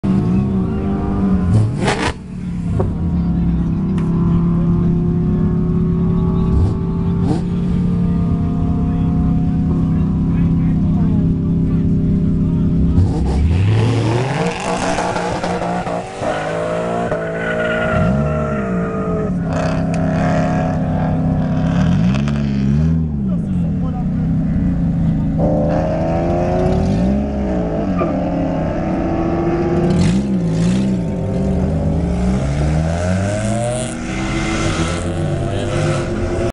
550 HP BMW M5 E39 Kompressor Sound + Powerslide & 545 HP Mercedes C 63 AMG
Runs Clastres de la Team Zone Rouge 02 le 19 octobre 2014